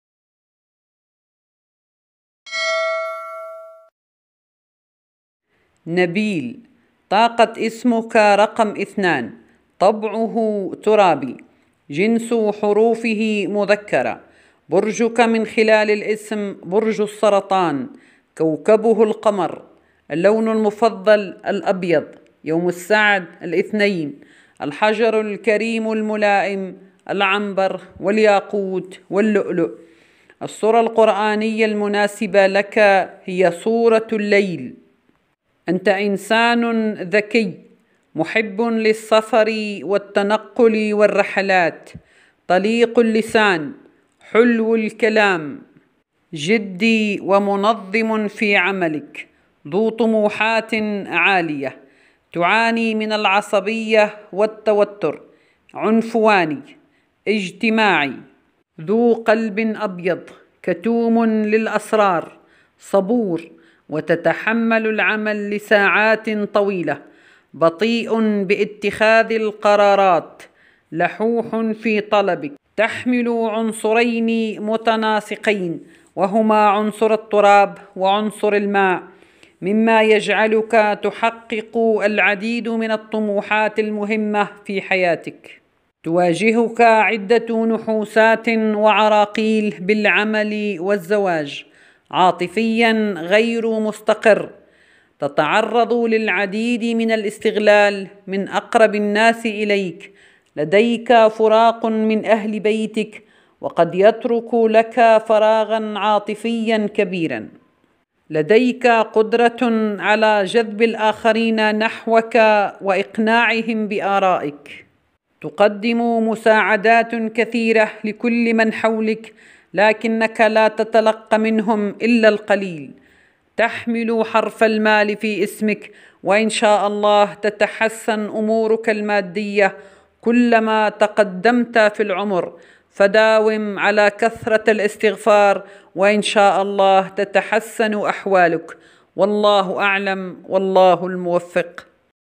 تحليل إسم#نبيل
فيديو يدرس ويحلل إسم#نبيل#صفاته#طباعه